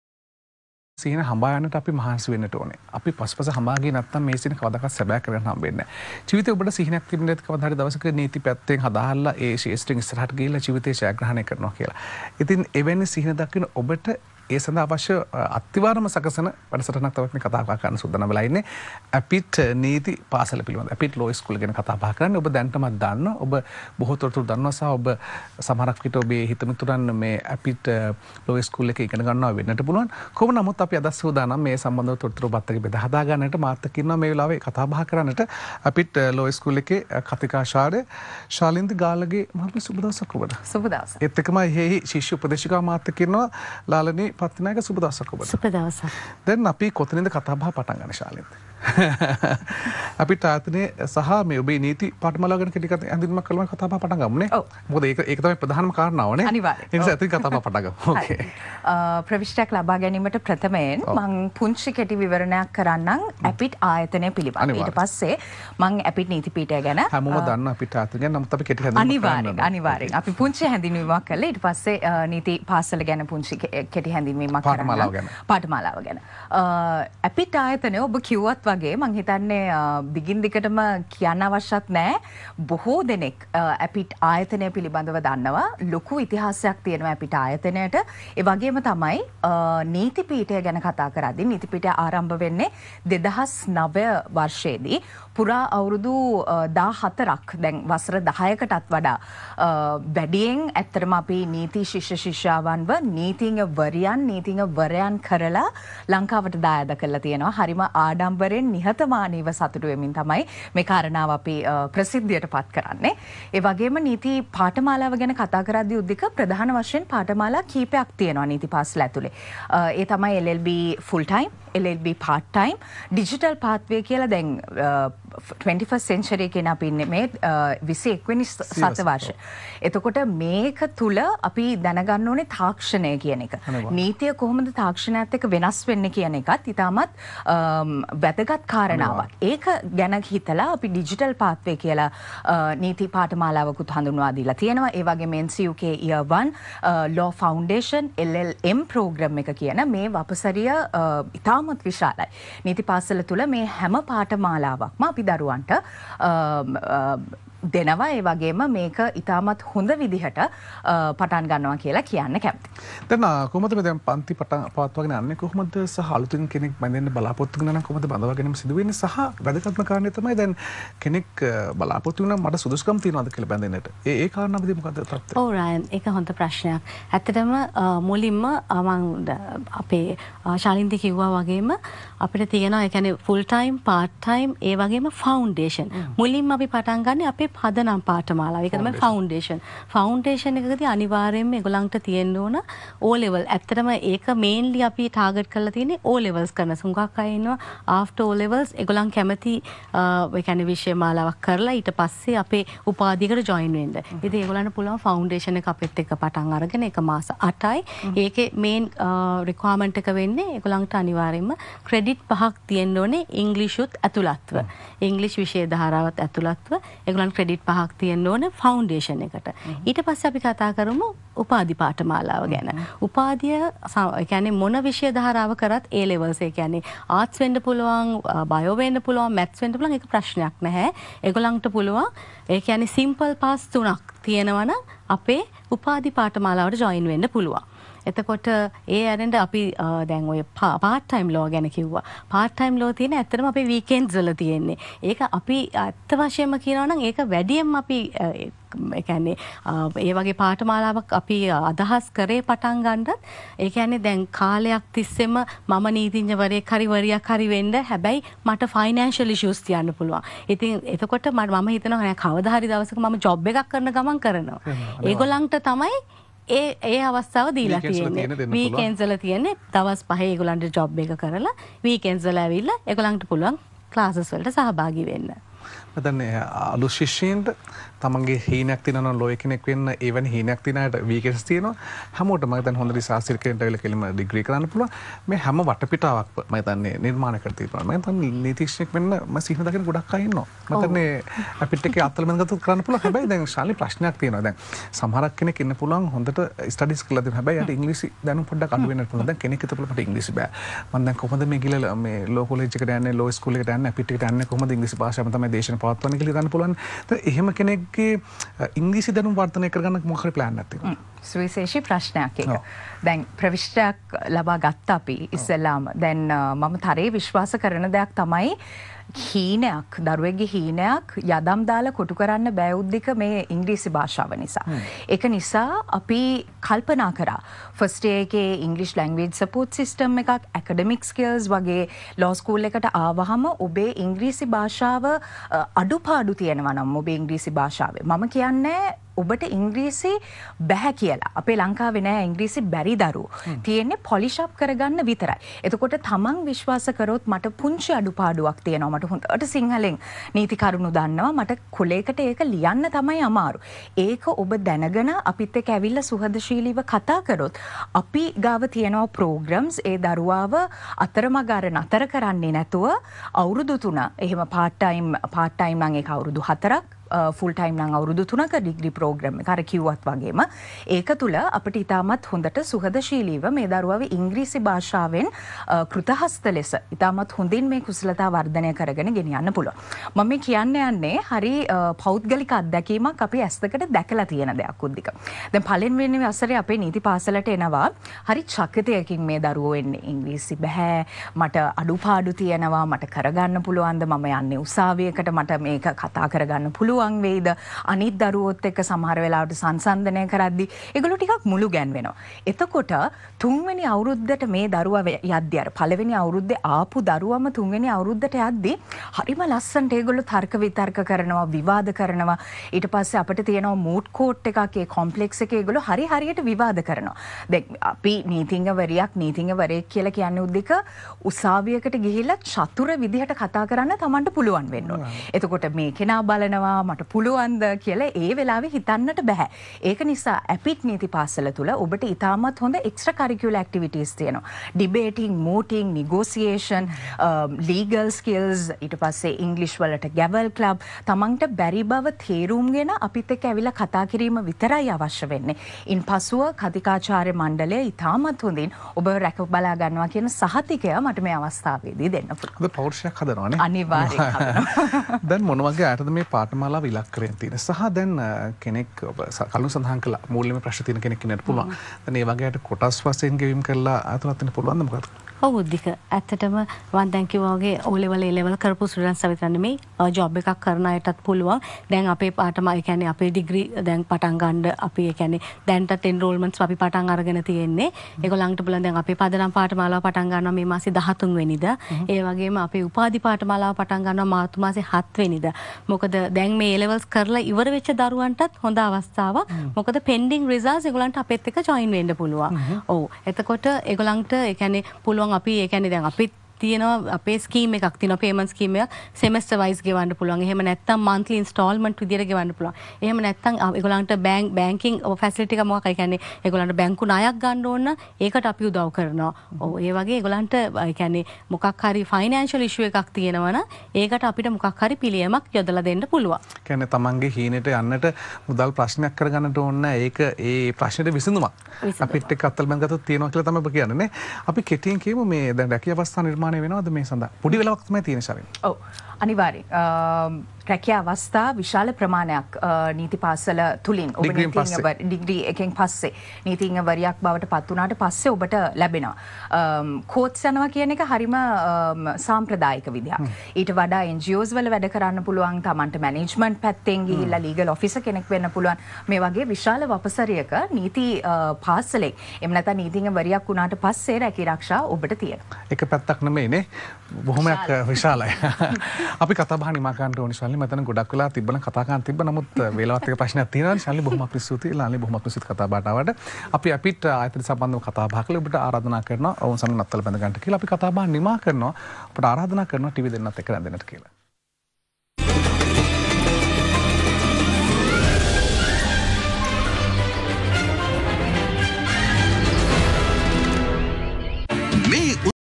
Tv Talk Show on Tv Derana – Law School, APIIT – 09.02.23
Ms. Shalindi Gallage – Lecturer, Law School, APIIT
Ms. Lalani Pathinayake – Marketing Executive - Student enrolment, APIIT